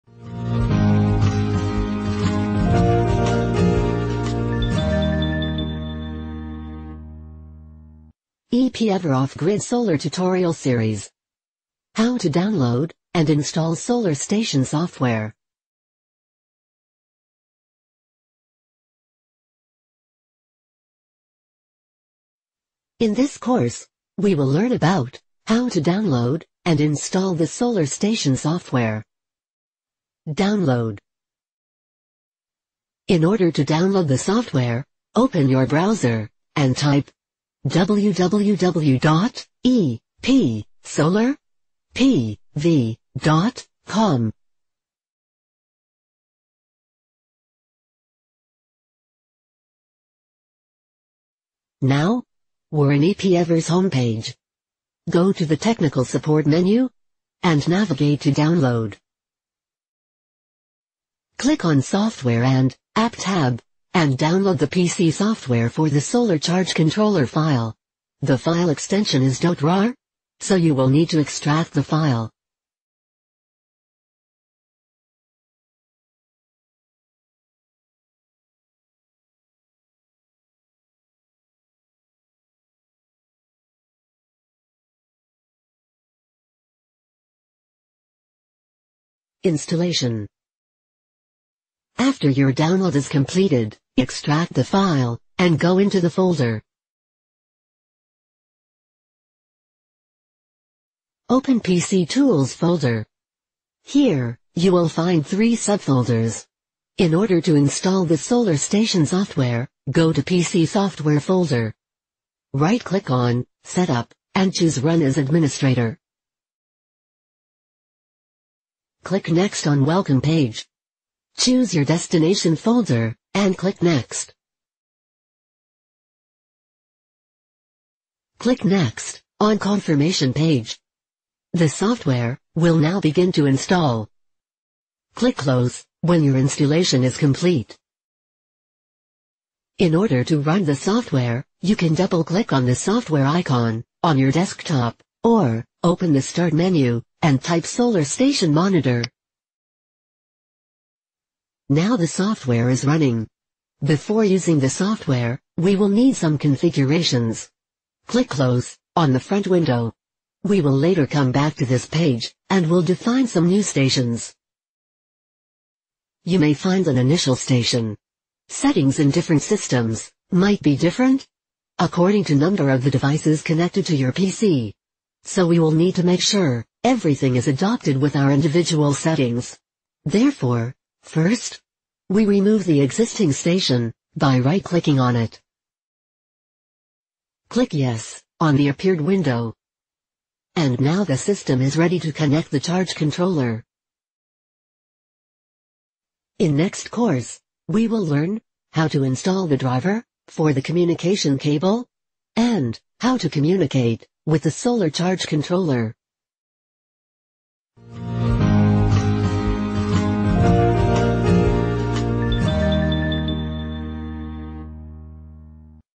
How to download and install "Solar Station" software
Video Caption :
In this course, we will learn about “How to download and Install the Solar Station Software”.
1. Download
In order to download the software, open your browser and type now we are in EPEVER’s home page.
Go to the “Technical support” menu, and navigate to “Download”.
Click on “Software and App” tab, and download the “PC Software for the Solar Charge Controller” file. The file extension is “.rar”, so you will need to extract the file.
2. Installation
After your download is completed, extract the file, and go into the folder.
Open “PCTools” folder.
Here you will find 3 subfolders. In order to install the solar station software, go to “PC software”.
Right click on “setup.exe”, and choose “Run as administrator”.
Click “Next” on welcome page
Choose your destination folder, and Click “Next”
Click “Next” of confirmation page
The software will now begin to install
Click “Close”, when your installation is complete.
In order to run the software, you can double click on the software icon on your desktop, or open the start menu, and type “solar station Monitor”
Now the software is running.
Before using the software, we will need some configurations.
Click close on the front window. We will later come back to this page and will define some new stations.
You may find an initial station. Settings in different systems might be different, according to the number of devices connected to your PC. So we will need to make sure, everything is adopted with our individual settings.
Therefore, first, we remove the existing station, by right-clicking on it.
Click “Yes” on the appeared window
And now the system is ready to “Connect the charge controller”. In the next course, we will learn how to install the driver for the communication cable, and “how to communicate with the solar charge controller”.

To access more related courses about off-grid solar systems please check EPEVER learning management system